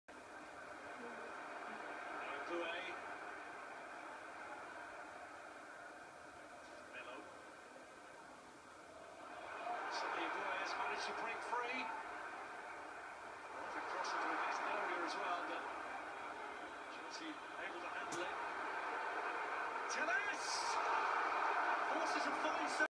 Felipe Melo (GAL) top players see passes that aren't even t
Brazilian Felipe Melo, one of the worlds creative defenders. Watch the value of having 4 threats up against the opposition back 4 and then he sees a pass that isn't really there...great a weariness and craft from a top international playing for galatasary in the UCL 2013/14